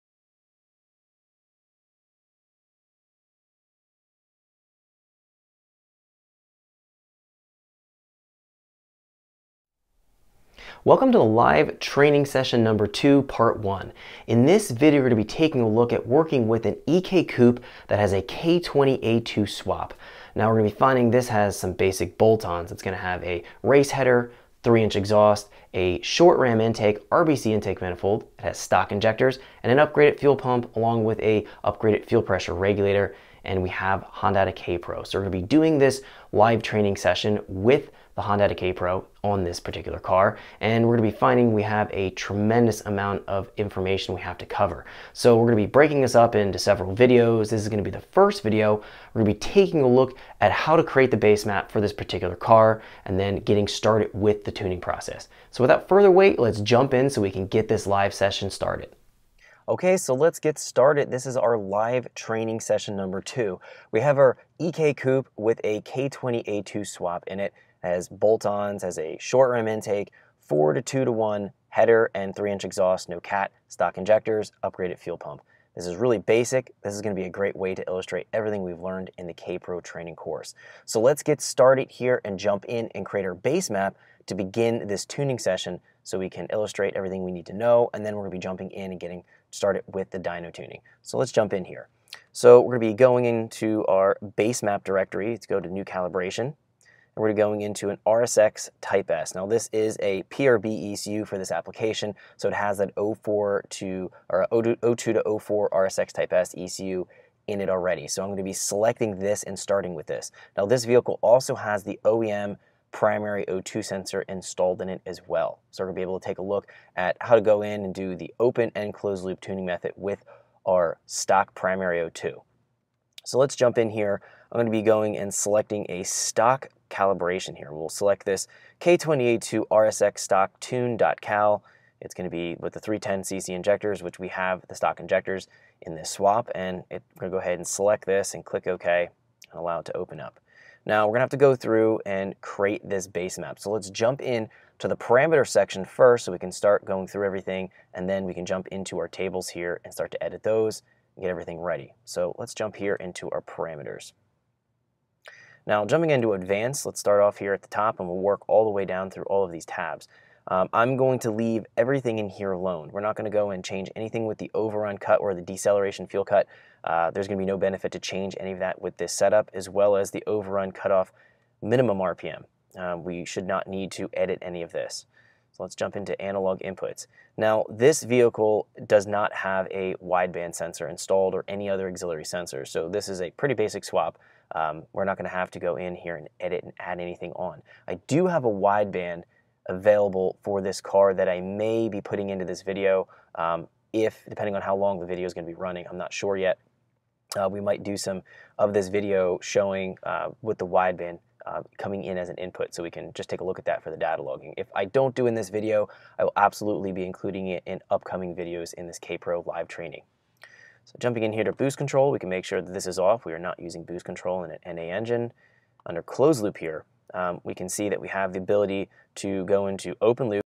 KPro Live Training: All Motor K20a2 EK Civic Part 1 | Evans Performance Academy
Follow along with lead instructor, Jeff Evans, as he walks you through a live training session on our in house chassis dyno.  We take a EK Civic with a K20a2 swap and tune it using a Hondata Kpro system.